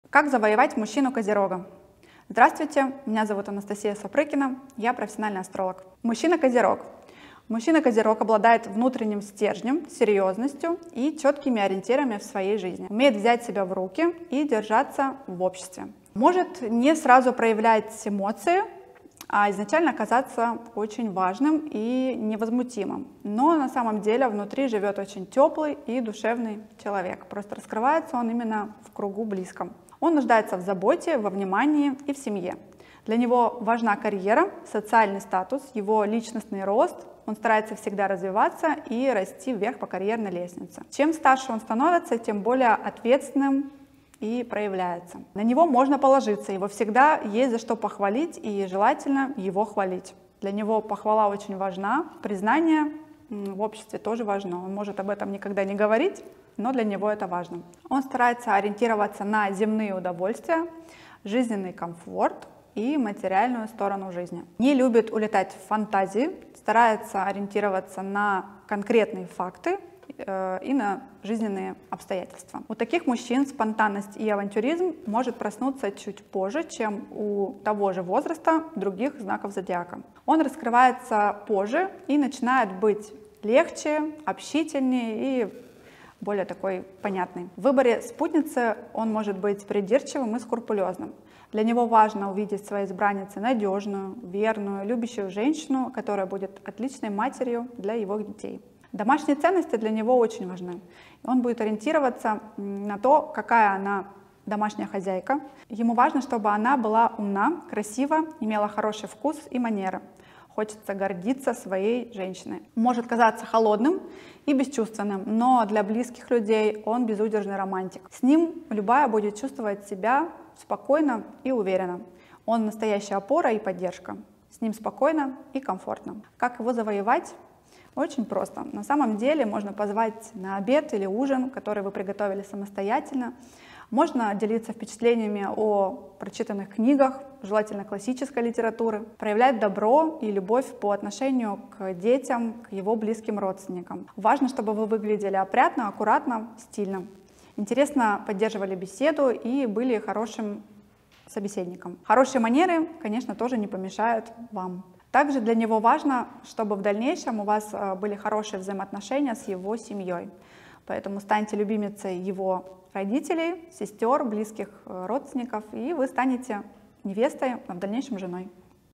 Как завоевать мужчину-Козерога: совет астролога
Как завоевать мужчину-Козерога рассказывает астролог Анастасия Сапрыкина. Смотрите в нашем видео!